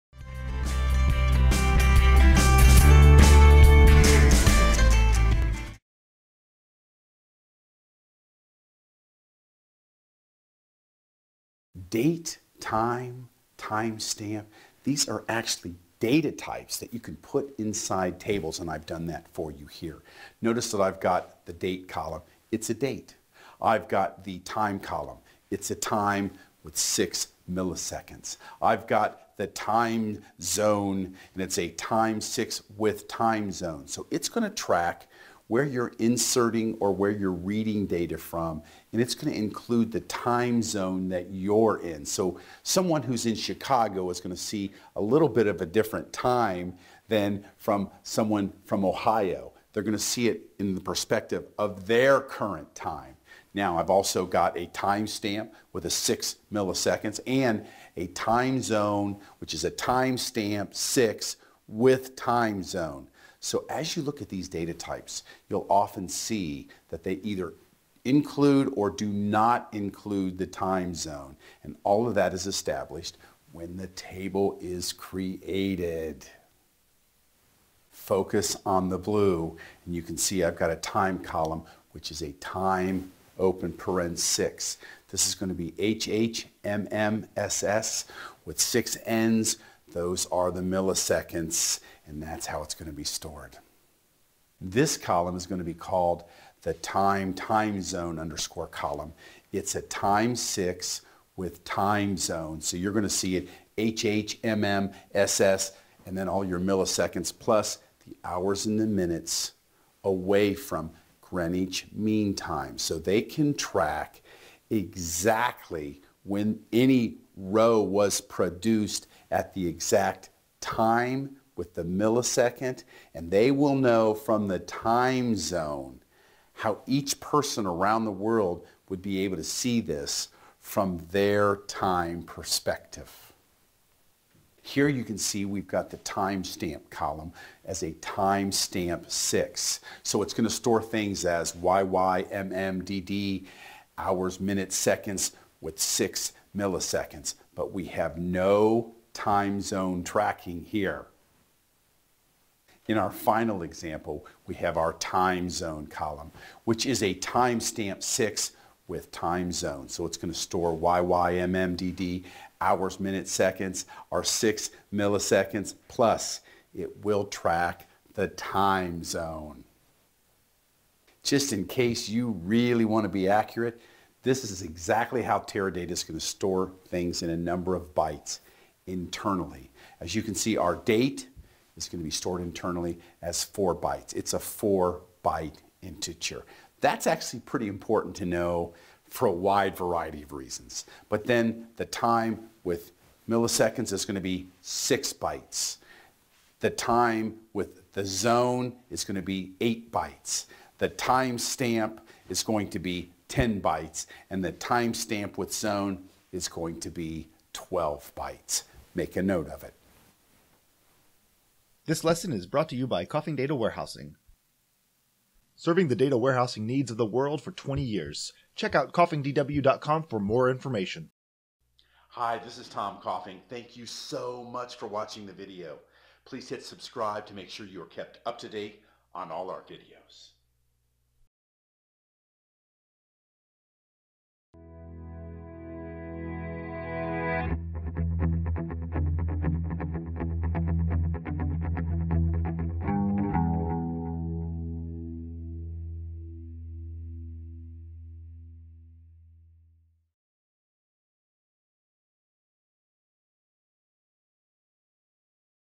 Let's Learn SQL! Lesson 44 : Storing Dates Internally in SQL
Explore SQL with Tom Coffing of Coffing Data Warehousing! In this lesson, learn how you store dates internally in SQL!